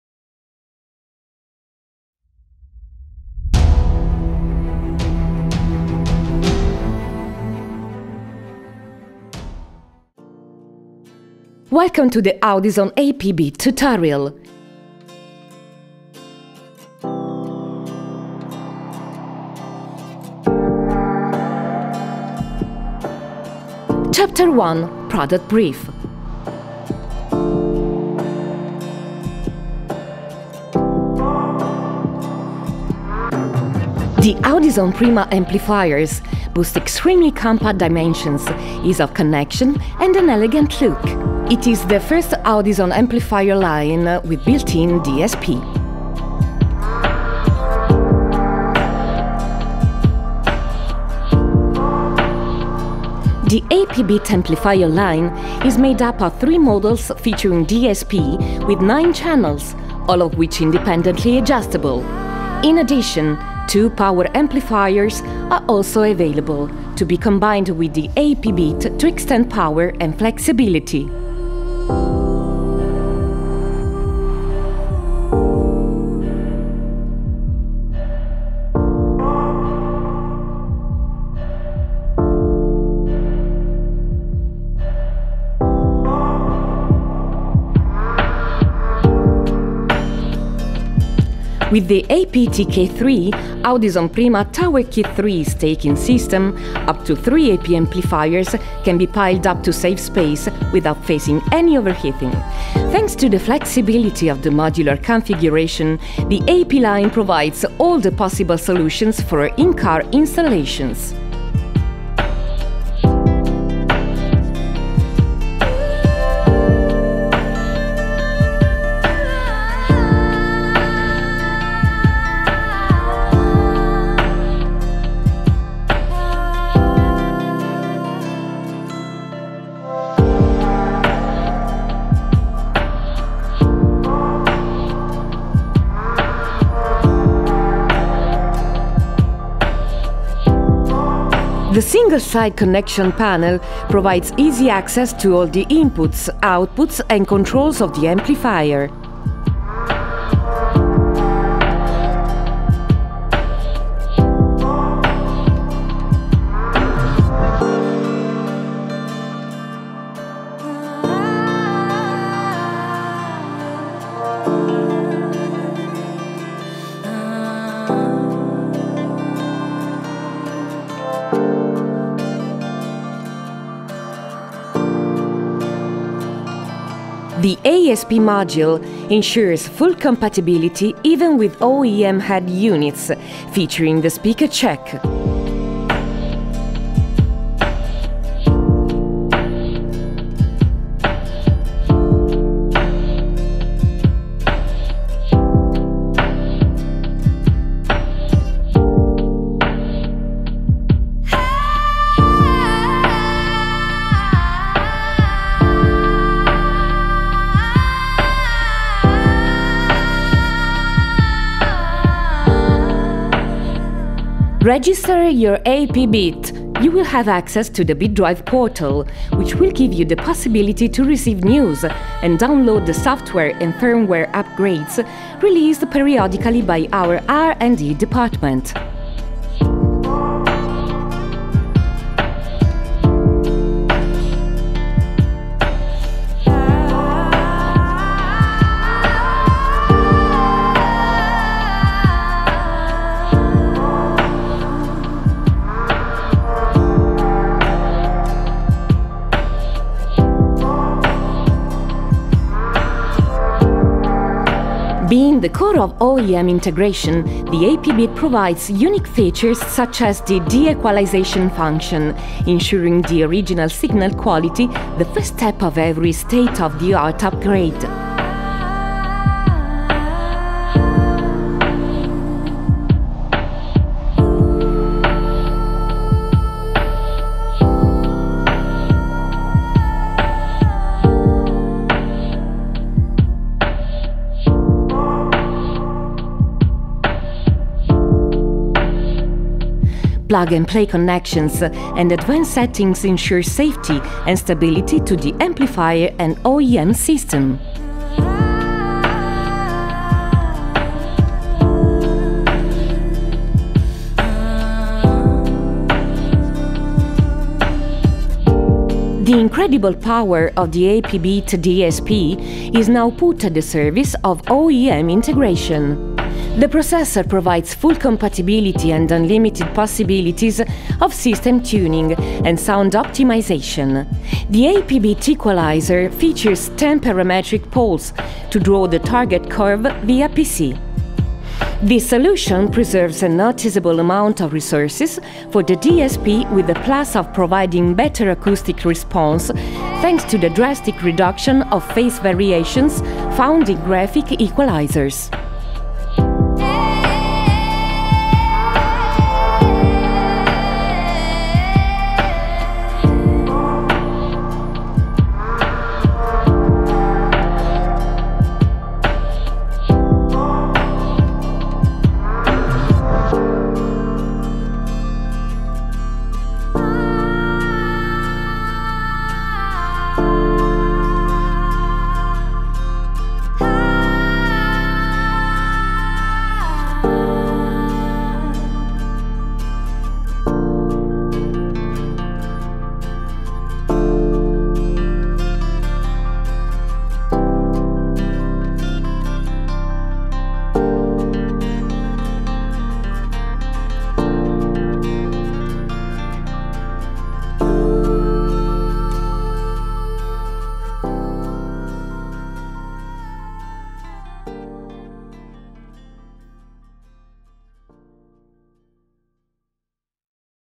1 - Audison Prima tutorial - Product Brief
Welcome to the Audison AP bit tutorial, Chapter 1 -Product Brief
 The Audison Prima amplifiers boast extremely compact dimensions, ease of connection and an elegant look. It is the first Audison amplifier line with built-in DSP
The AP bit amplifier line is made up of three models featuring DSP with 9 channels, all of which independently adjustable. In addition, two power amplifiers are also available, to be combined with the AP bit to extend power and flexibility